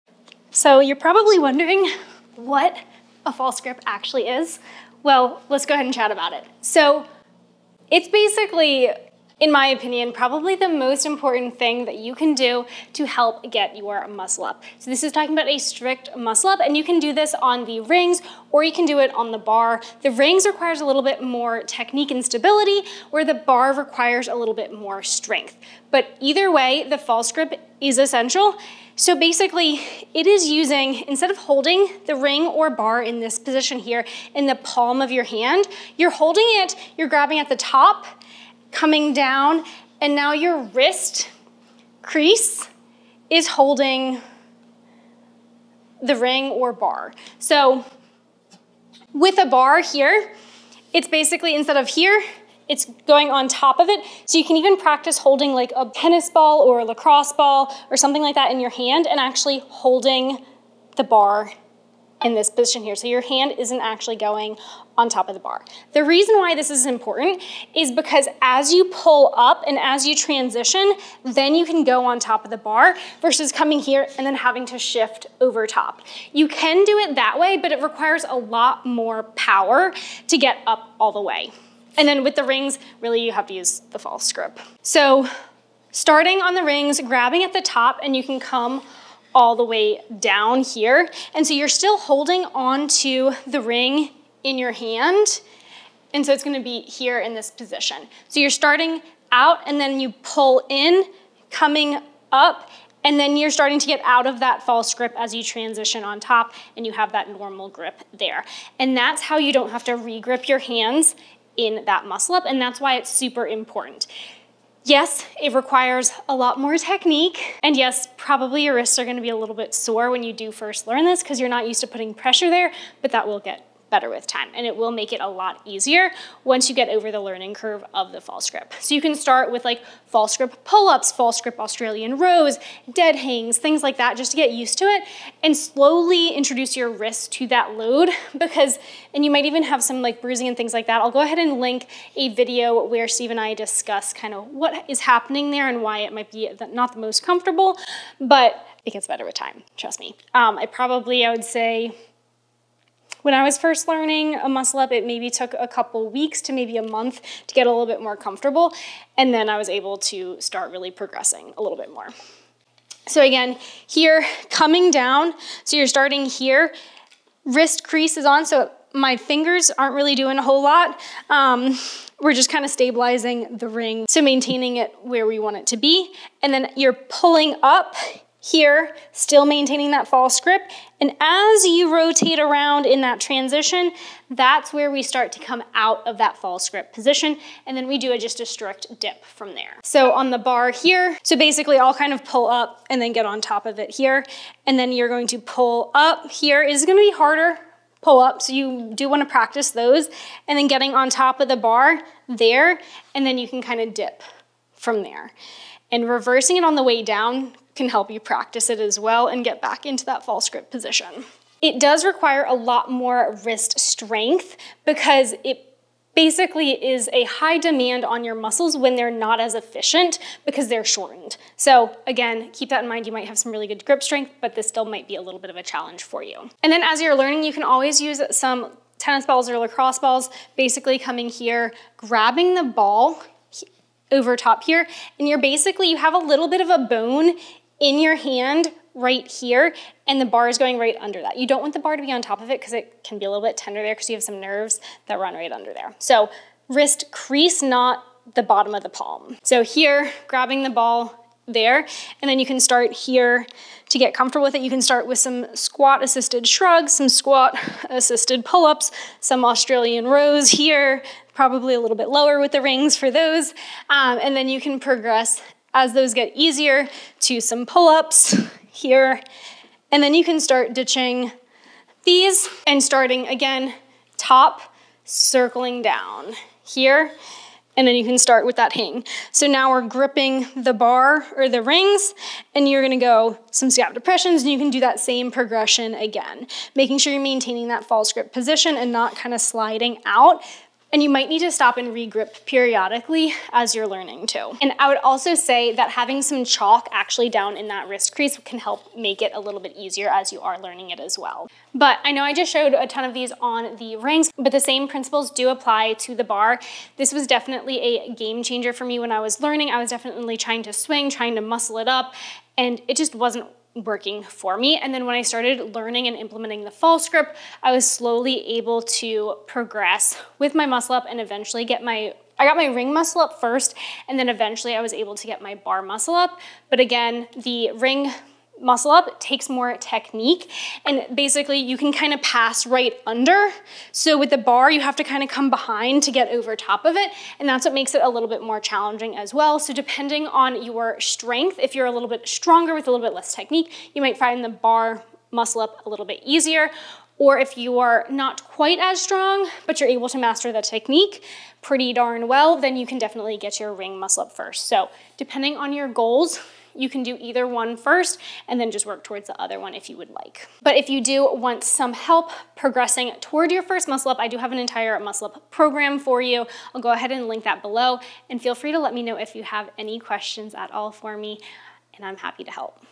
False grip: the secret to a strict muscle up
So you may be wondering how to do a muscle up or what muscle up technique to use as a beginner learning a slow muscle up – the secret I personally found helpful and the muscle up technique I teach is using the false grip.

The false grip muscle up helps you transition of the bar or rings without having to adjust the hand placement, making the muscle up transition smooth.

If you want more drills/exercises to work on your muscle up, check out my muscle up program and my other programs in my app

Chapters:
0:00 - Intro
0:35 -  What is the false grip
1:05 - Why the false grip
1:54 - What to expect
2:42 - Ring false grip
3:09 - Bar false grip
3:31 - False grip vs. standard grip
3:46 - False grip drills
5:01 - Ring vs. Bar muscle up
6:00 - Muscle up program